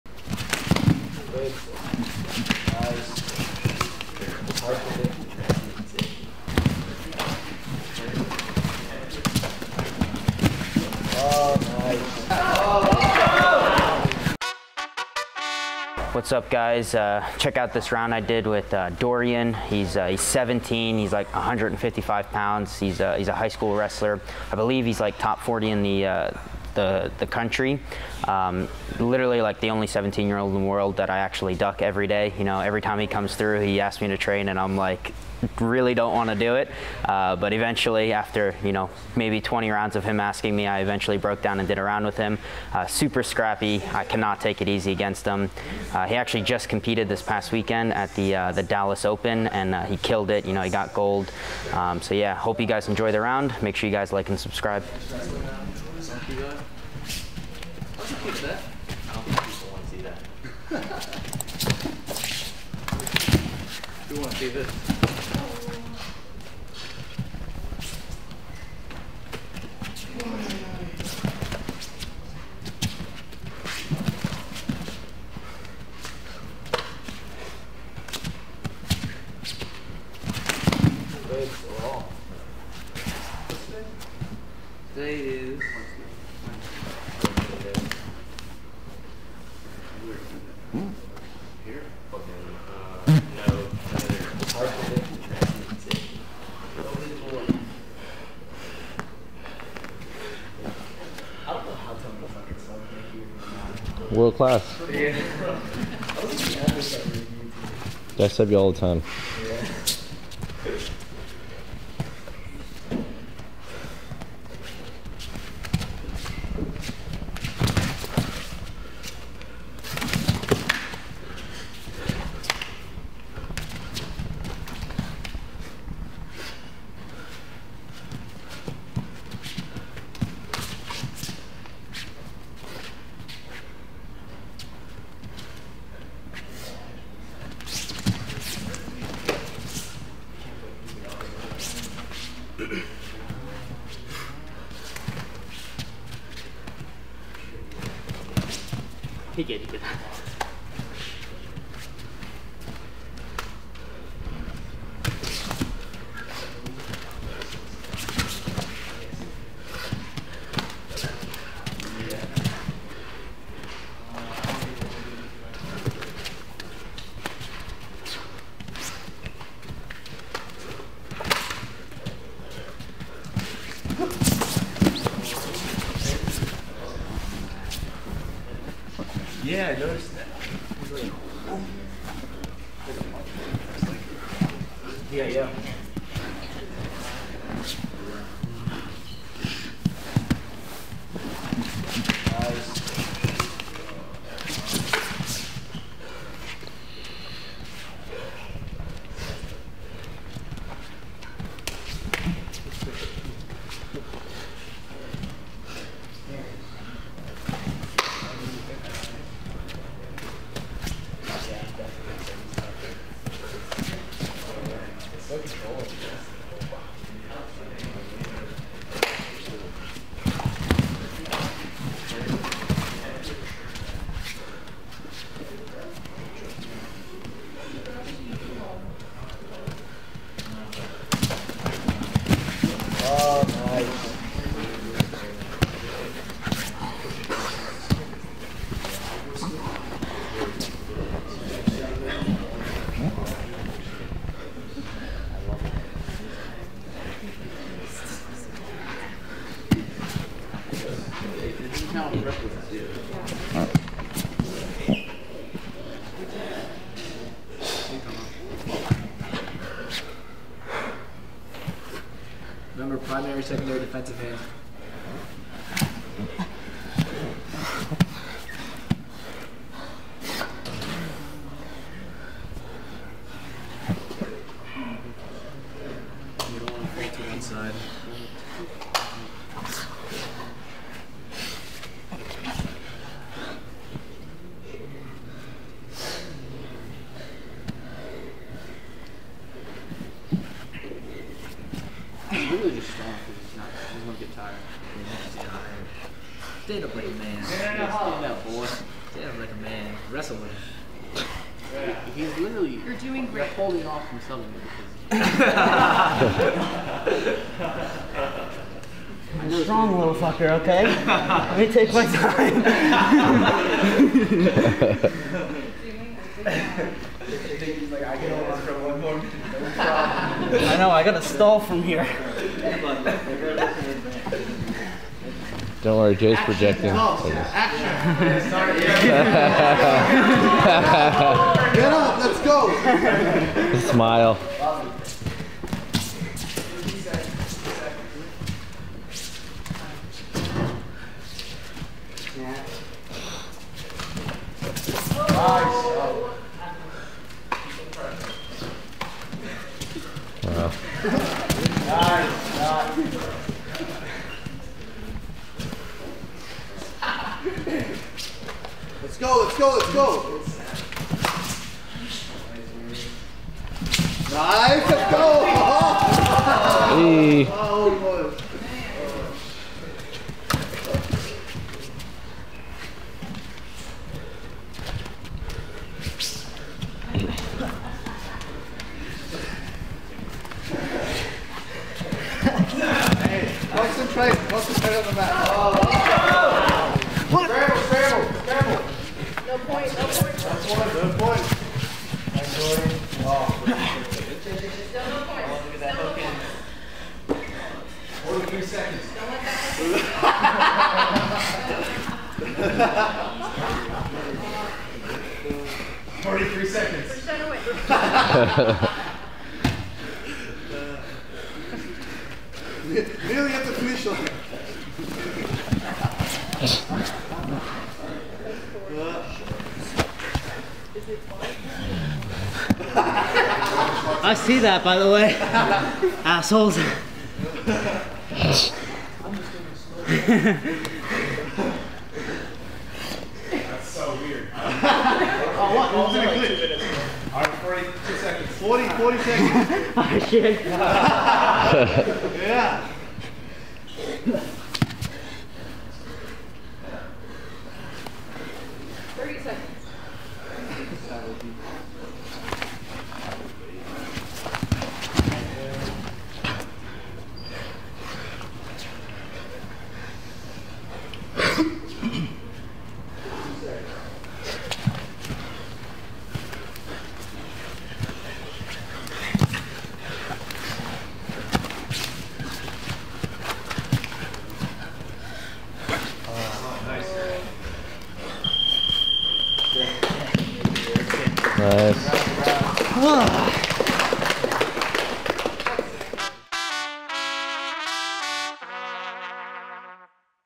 Scrappy! 17yo ADCC Open Champ Comes After Nicky Ryan | B-Team Training
Merch out now! | 17-year-old Dorian Olivarez is back! He's well-known at B-Team as the kid nobody wants to roll with, as every round with him is a war. A nationally ranked high school wrestler and grappling competitior, Dorian is coming hot off winning gold in the stacked -65kg division at the ADCC Dallas Open. Enjoy this round of Nicky Ryan vs Dorian Olivarez!